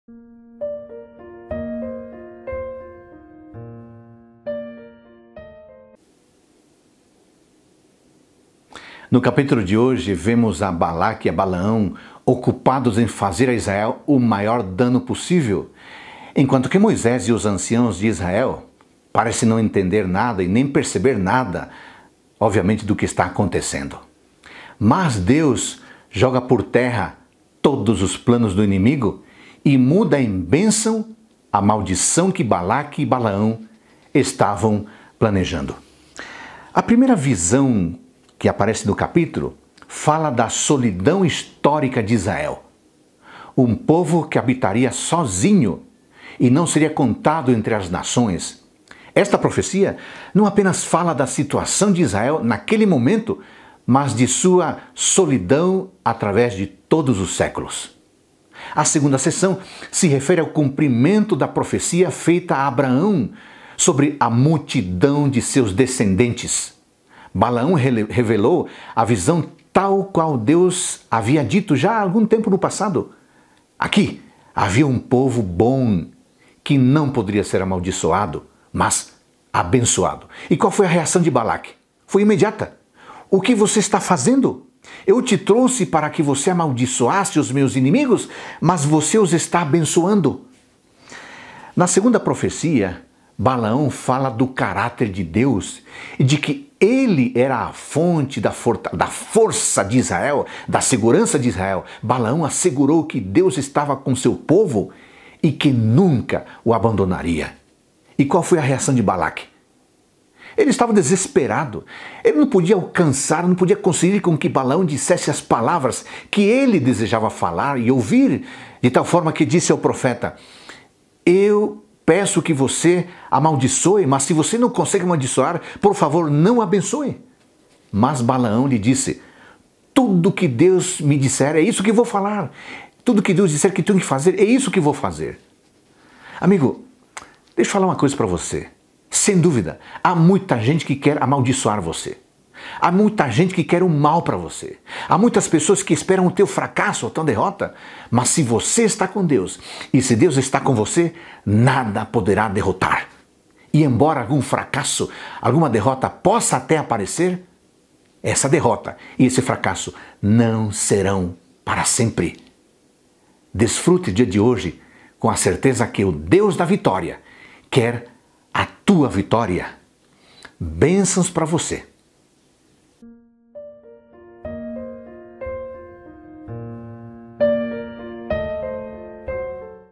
Números 23 - Reavivados por Sua Palavra
Certamente há pessoas que te desejam o mal e torcem por teu fracasso. Mas se estás com Deus e Ele contigo, nada nem ninguém pode te derrotar. E ainda que venha algum fracasso, não será para sempre.